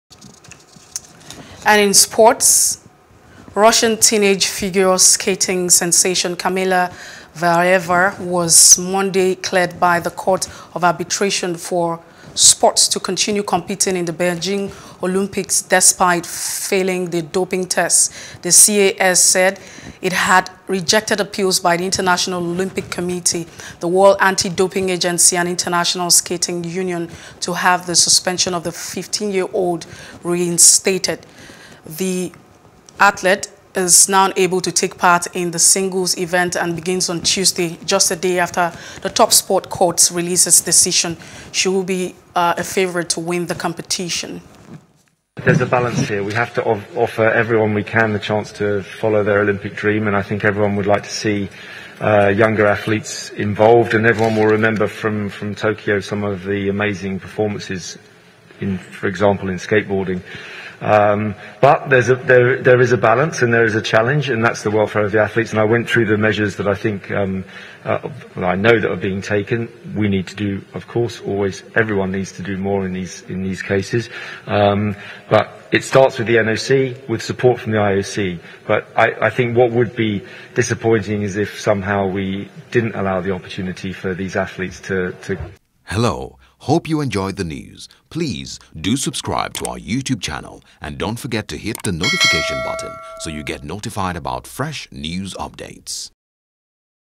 Olympics: Russian Skater Valieva Cleared To Continue In Beijing : C.A.S. | SPORTS
Russian teenage figure skating sensation Kamila Valieva was on Monday cleared by the Court of Arbitration for Sport to continue competing in the Beijing Olympics despite failing a doping test.